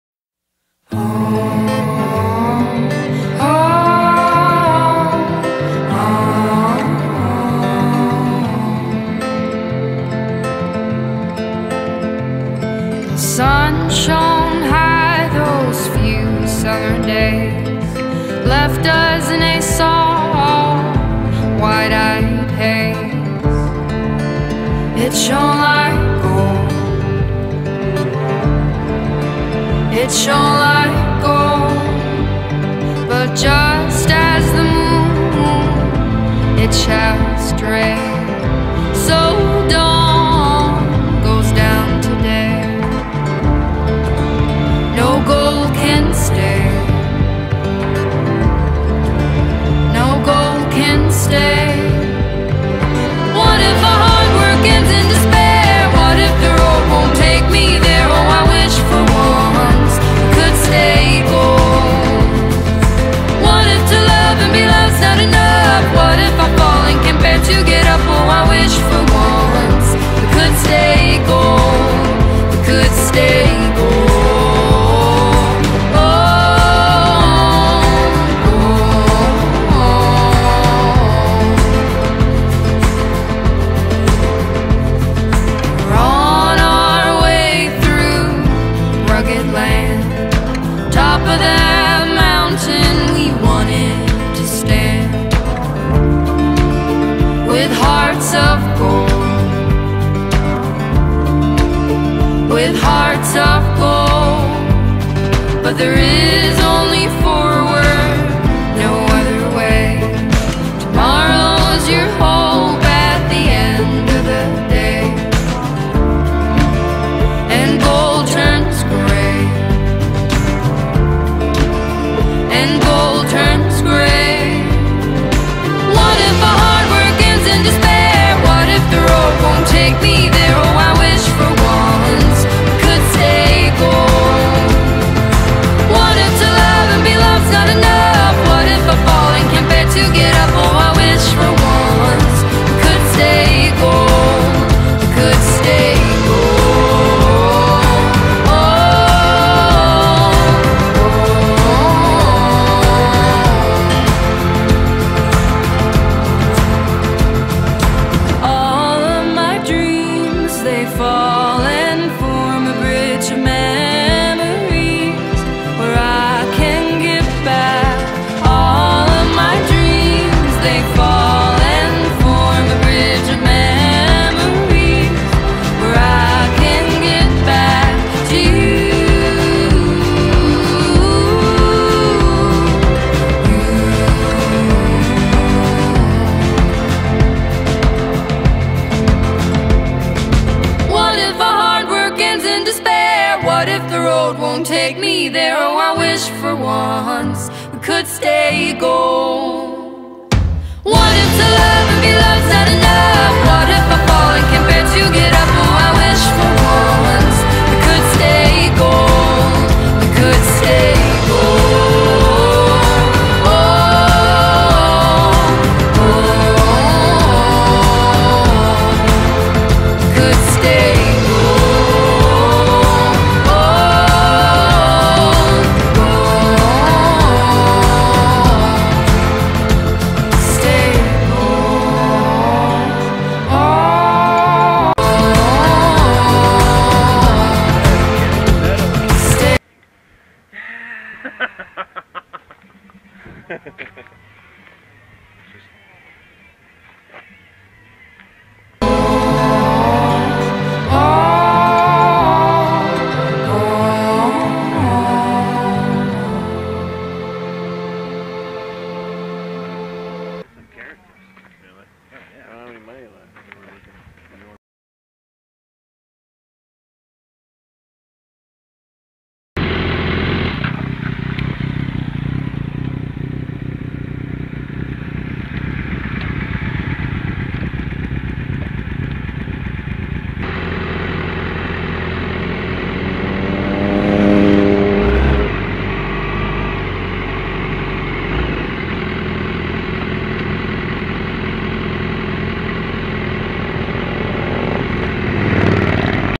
Sequoia National Forest May 2015
David collin and jamie ride sequoia national forest.